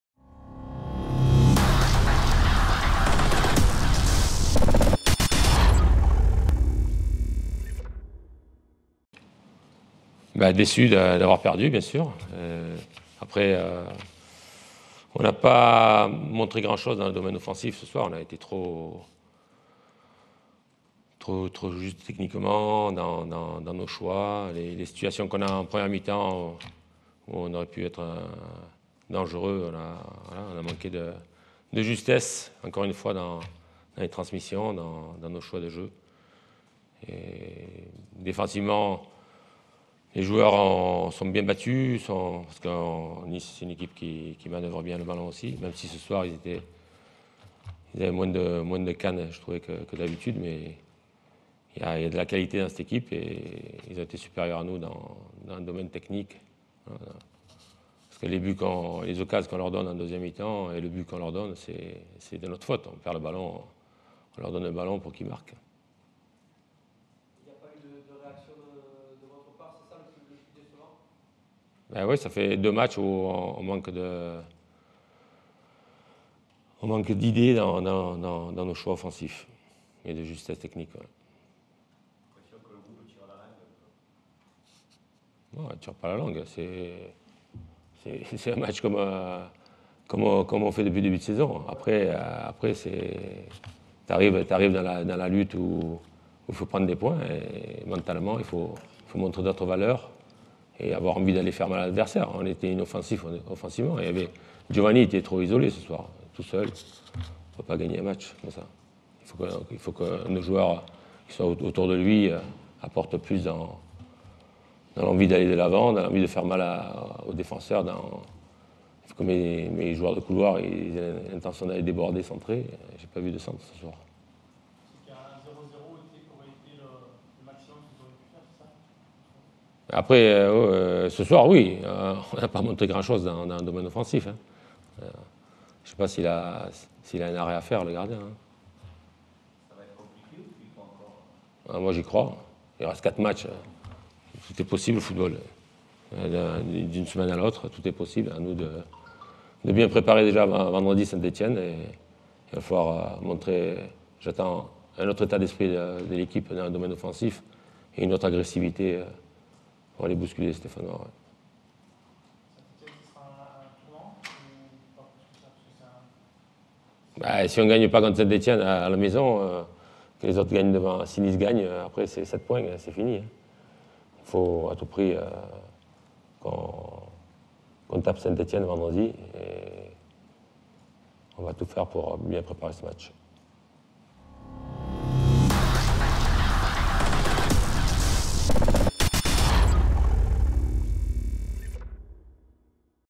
Michel Der Zakarian après OGCN 1-0 MHSC
Le coach Michel Der Zakarian en conférence de presse d'après-match à l'Allianz Riviera de Nice ce dimanche en fin d'après-midi (OGC Nice 1-0 MHSC, 34ème journée Ligue 1 Conforama).